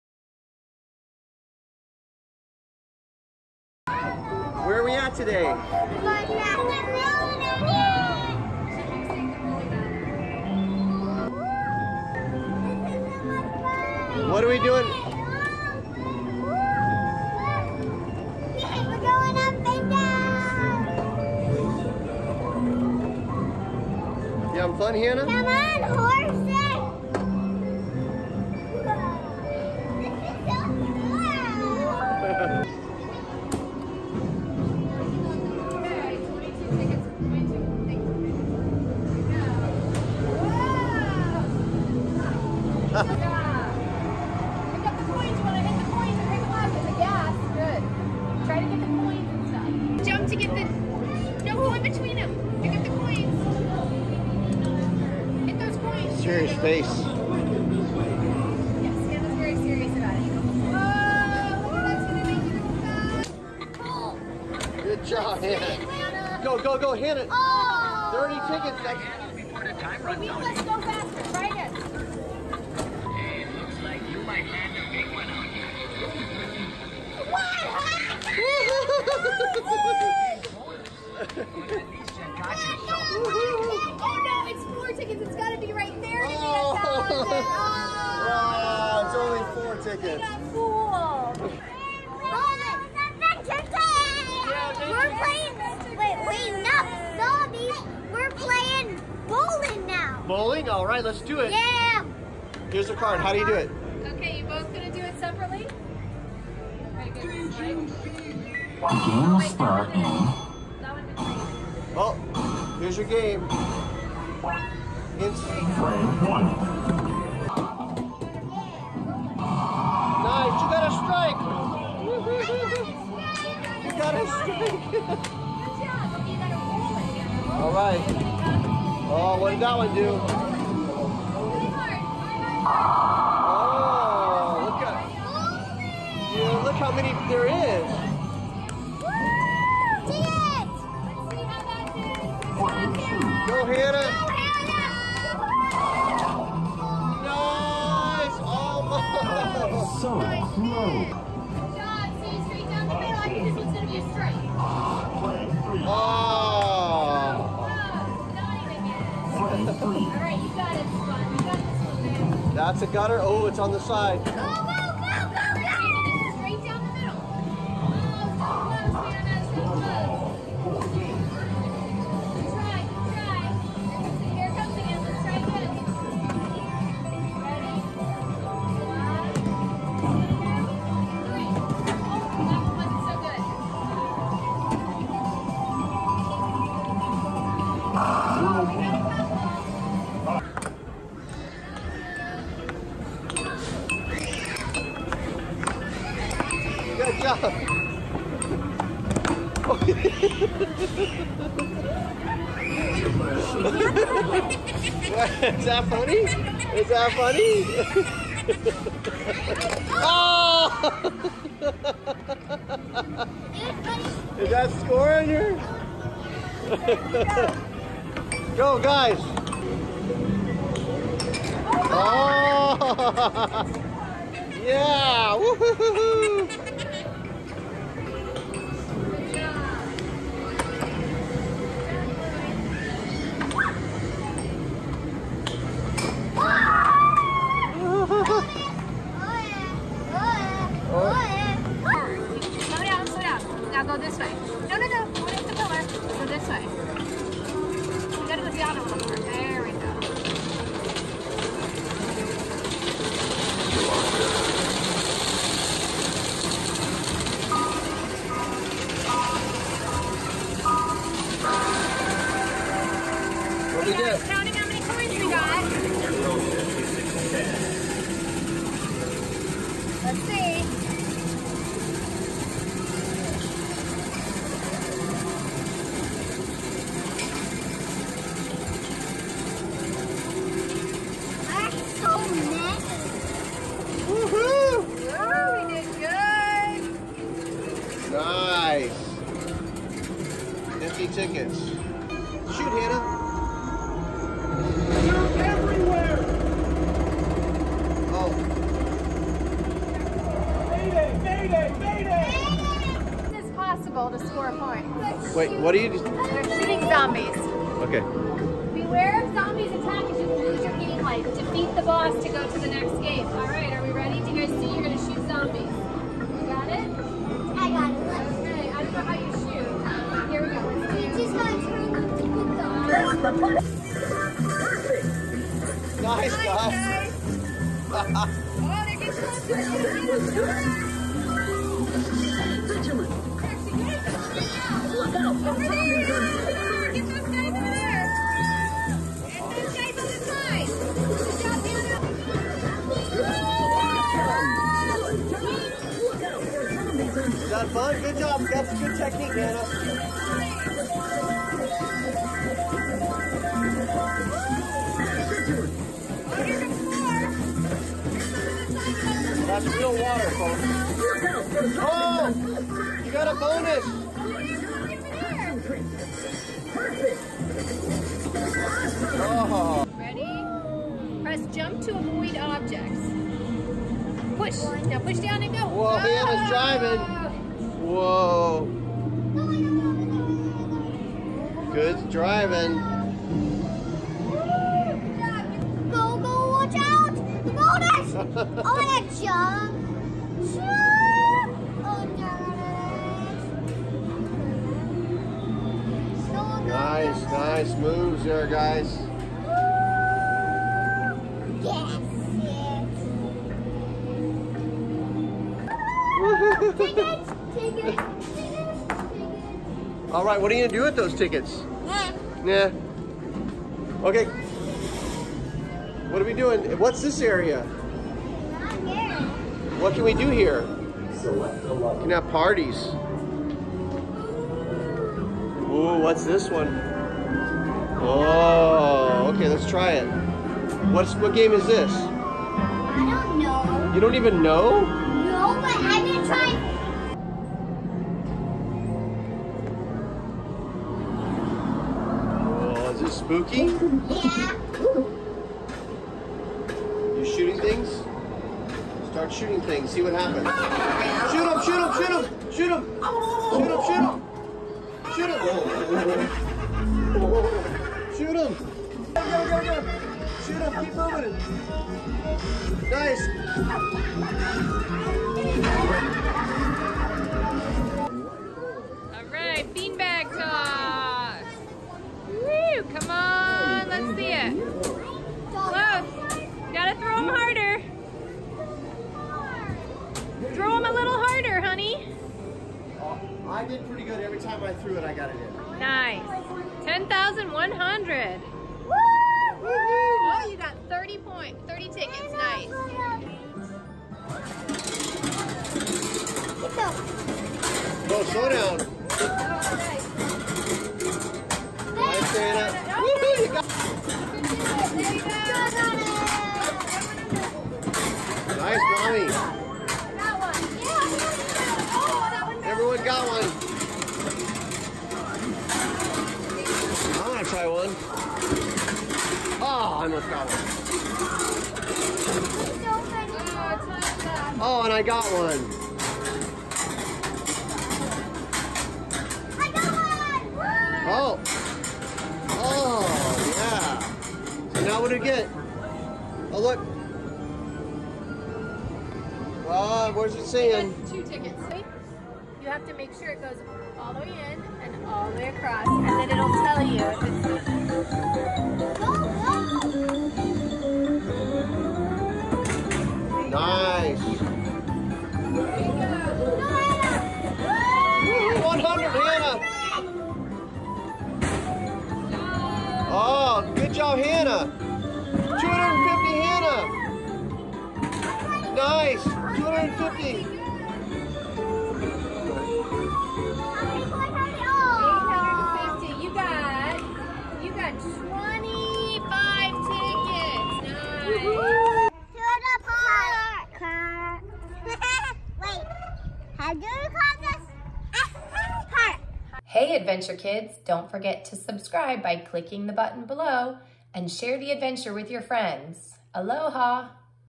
ARCADE GAMES at FUN FACTORY!! - Adventure Kids Hawaii
Chloe and Hannah experience the fun of arcade games at the Fun Factory! Join them for arcade bowling, ring toss, merry-go-round and more!!